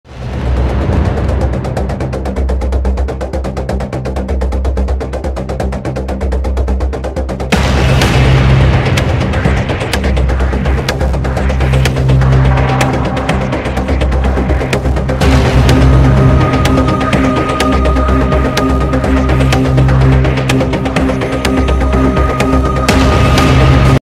Ibrahimović VS Lukaku fight - Ac Milan - Inter Mediolan The Coppa Italia red card
Todays fight ibra lukaku red card